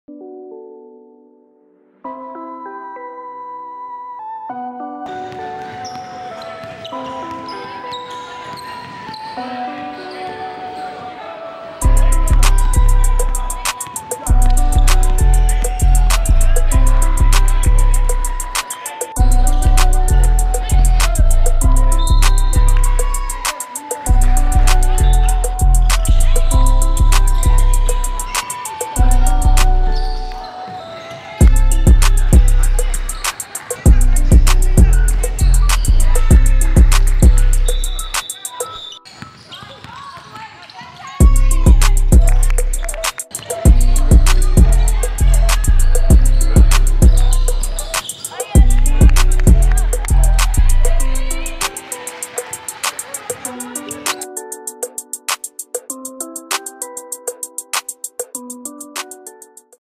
Coniah Cooley -- 2019 Check Me Out Showcase Highlights
Coniah Cooley -- 2021 PG -- Jersey #17
Mansfield Legacy HS | Pro Skills
Check Me Out Showcase - Austin Texas - May 2019